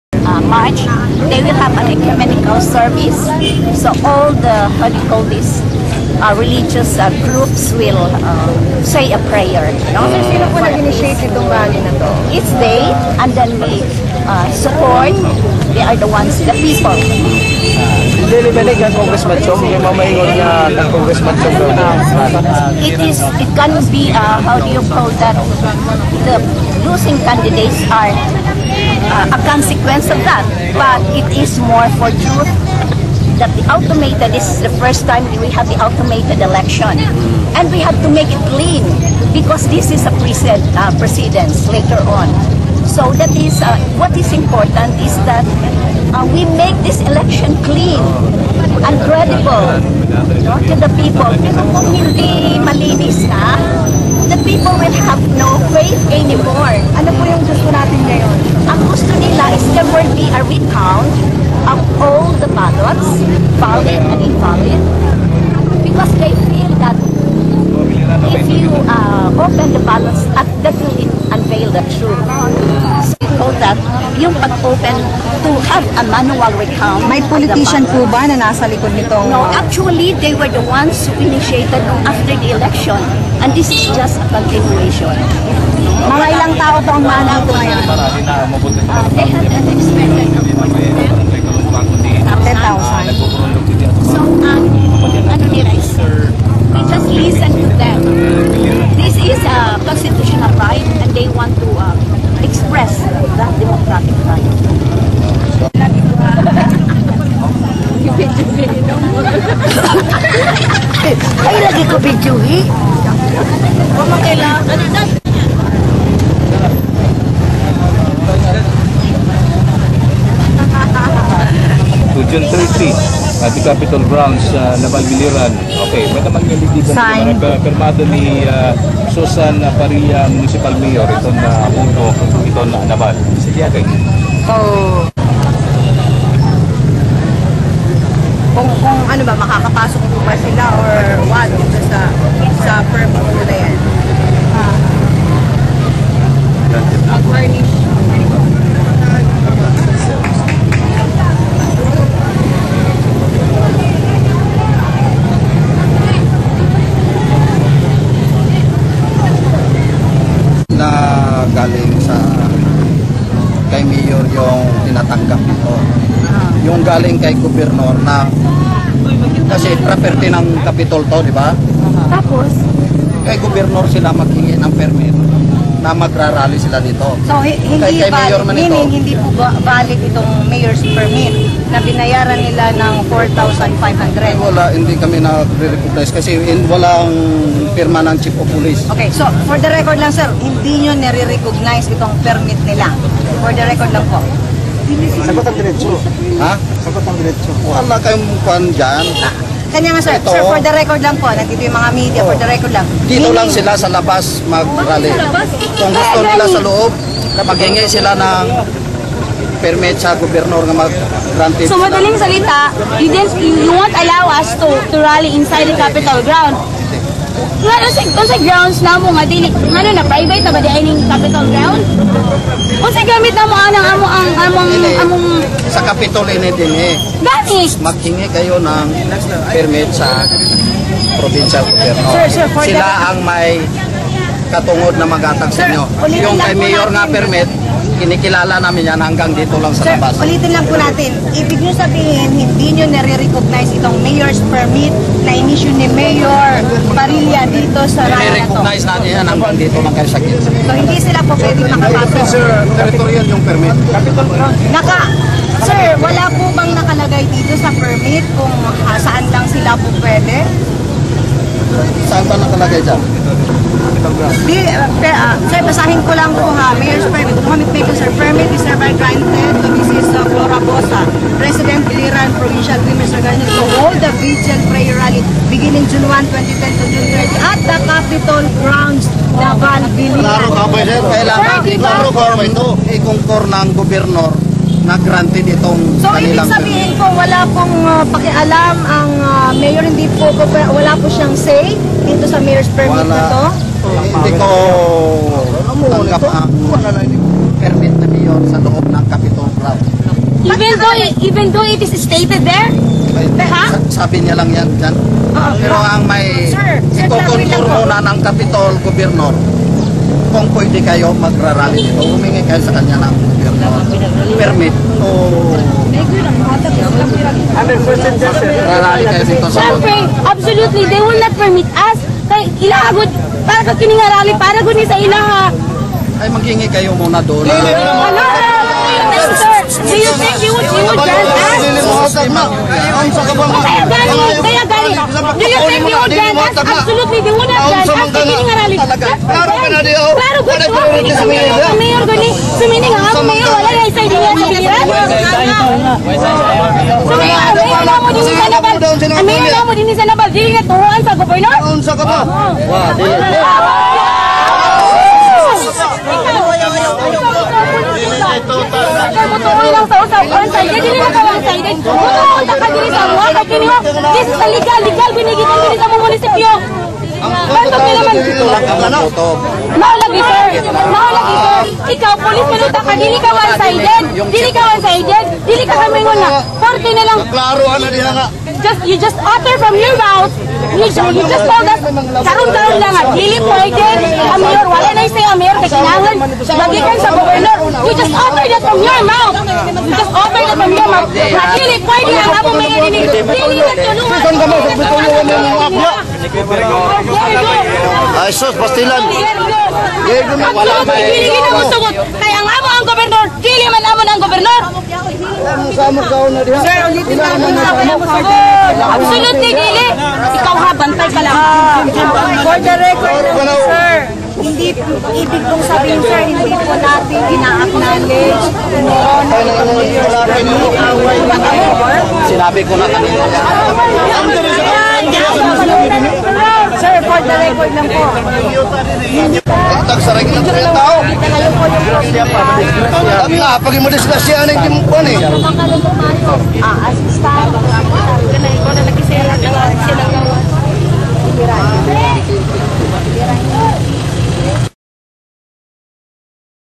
MGA SUPPORTER NI CHONG UG PULIS NAG LALIS...
Mga supporter ni Chong ug mga pulis nag lalis,sa dihang wala sila pasudla sa compound sa capitilyo..bisan pa aduna silay permit na ge isyu ni Mayor Susan Parilla...